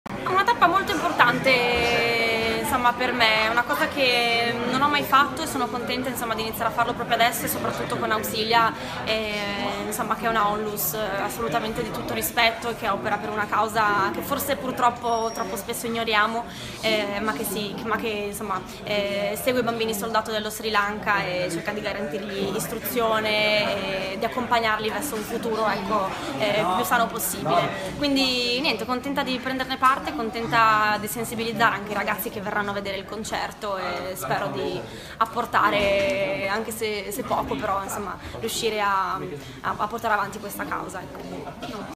LODOVICA COMELLO PER AUXILIA
Intervista a Lodovica Comello testimonial di @uxilia Onlus per la tutela dei bambini soldato